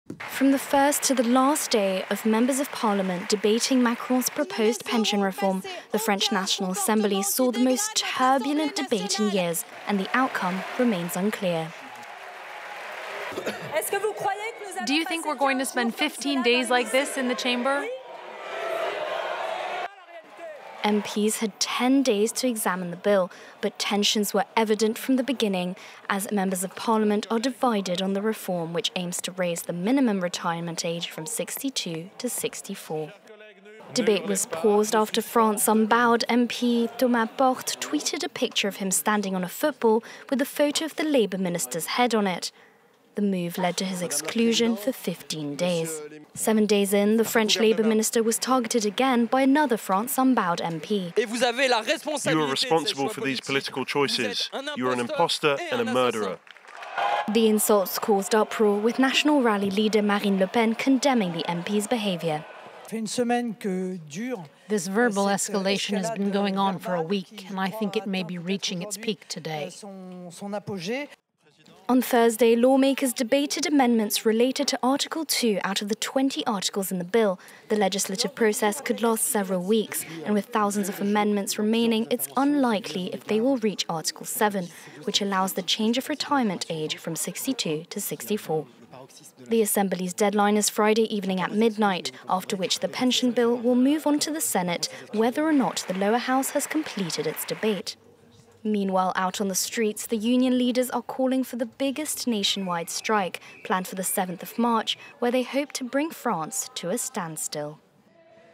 France's debate on contested pension bill to head to Senate • FRANCE 24 English
The lower house of France's parliament debated a contested pension bill aimed at raising the country's retirement age from 62 to 64 during what was expected to be lawmakers' last discussion on the proposal before sending the legislation to the Senate. President Emmanuel Macron's government has vowed to go ahead with the reform despite nationwide protests and strikes and opinion polls consistently showing a majority of French people oppose the change.